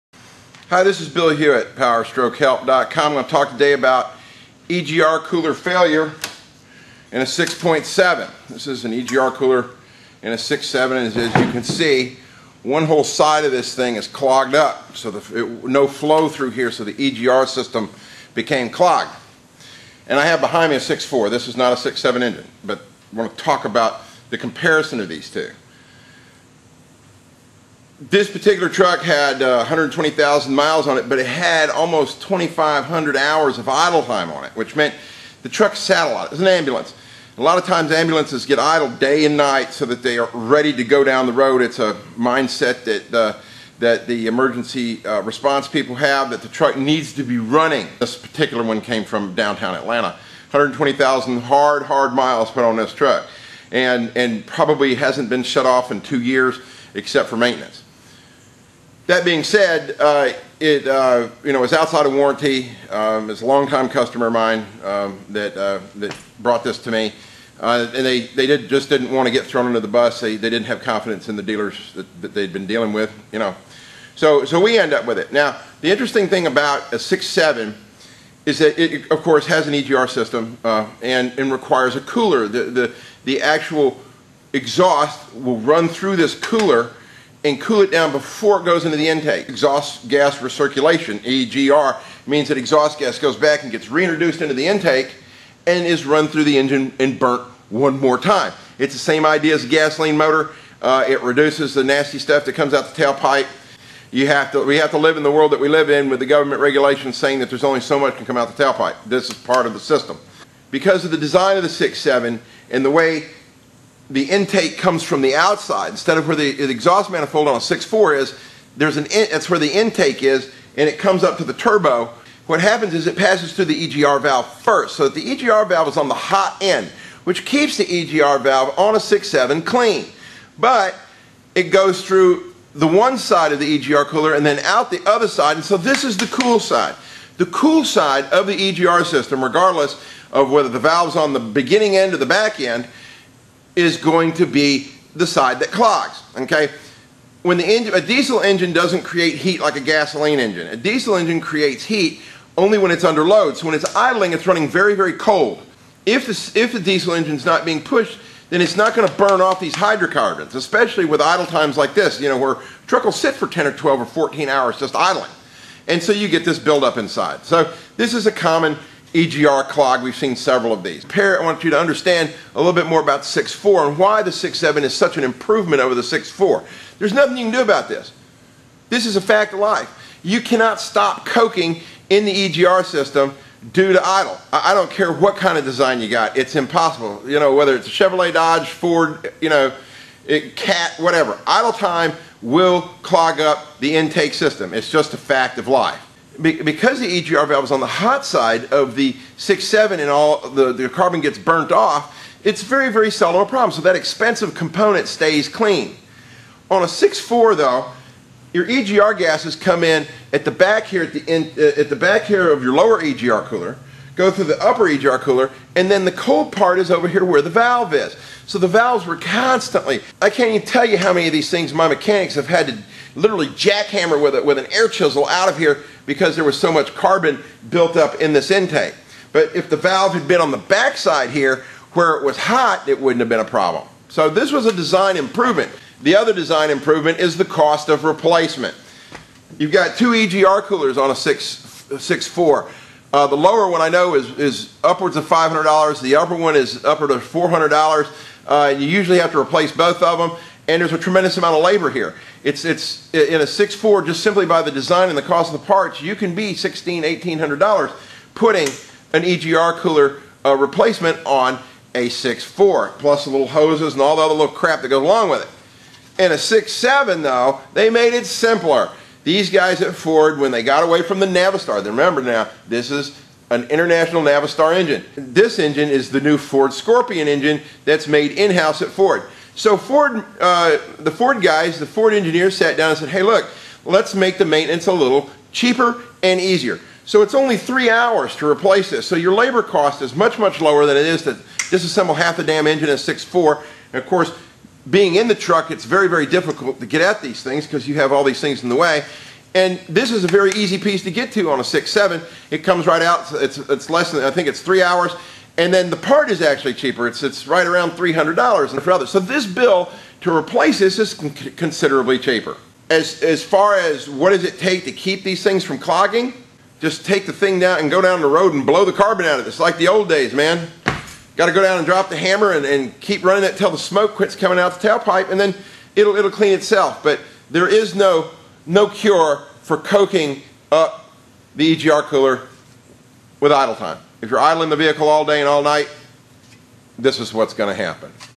6 7 EGR COOLER FAILURE
documentation of a 6.7L PowerStroke EGR cooler failure in an urban ambulance with high idle times.